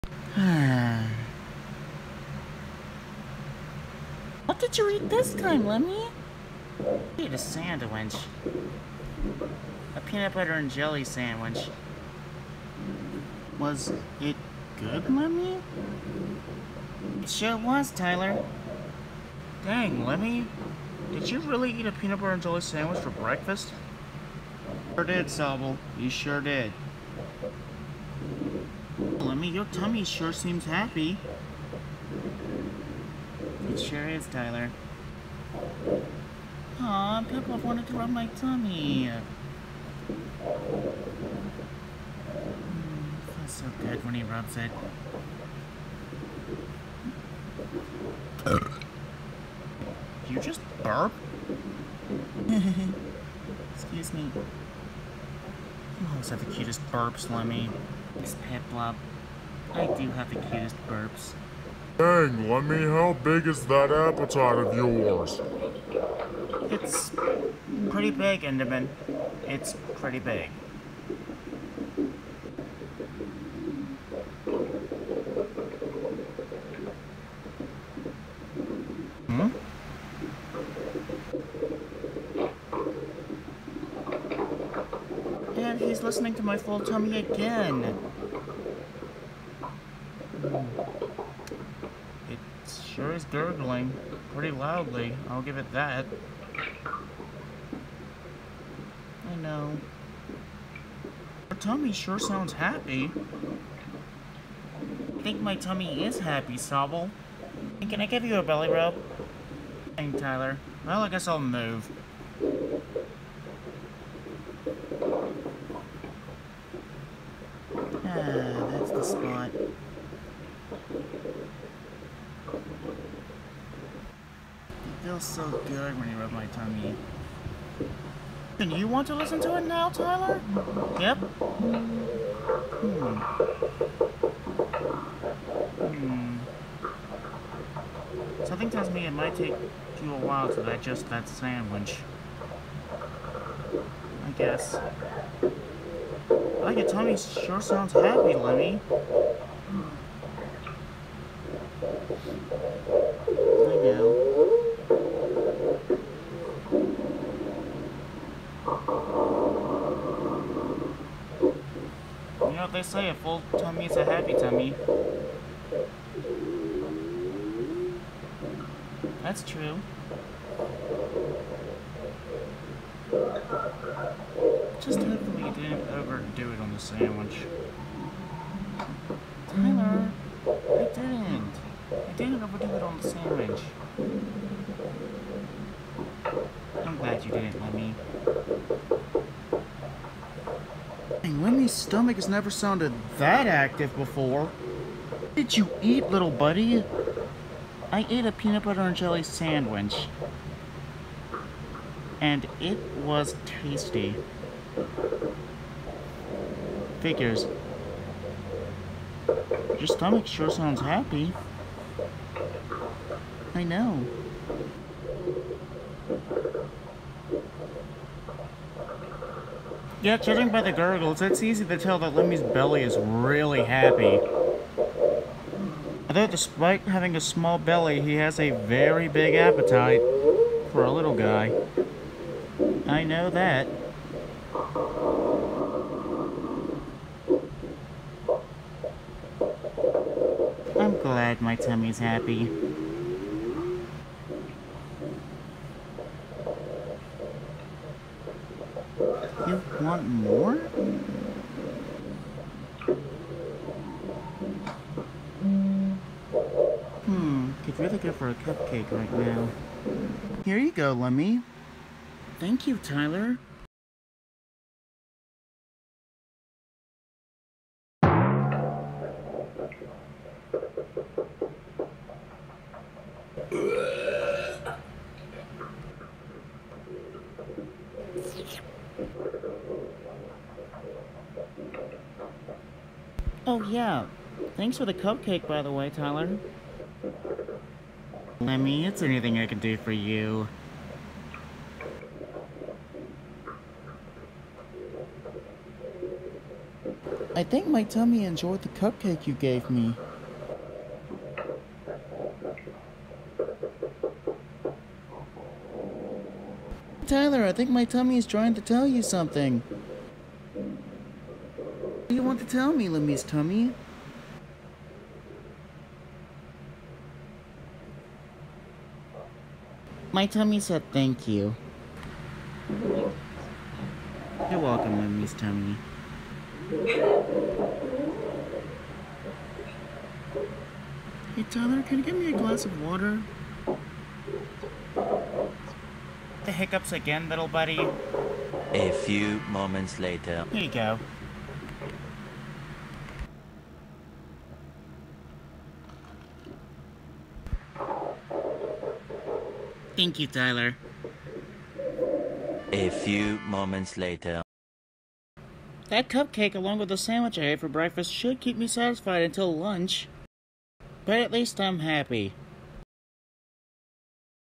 Lemmy’s tasty breakfast& Sweet snack+cute tummy gurgles&hiccups
Lemmy ate a Pb&j for breakfast and a cupcake for a snack after that giving lemmy a very happy tummy as well as the hiccups at the same time